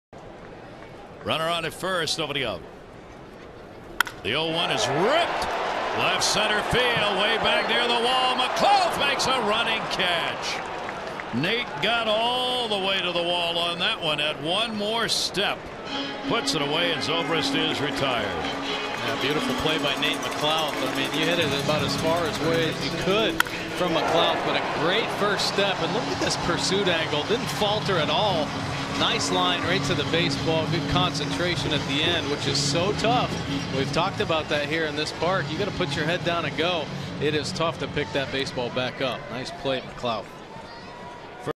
BAL@TB: McLouth runs down a fly ball to the track
9/23/13: Nate McLouth chases Ben Zobrist's deep fly ball to the track in left-center and makes a great running catch for the out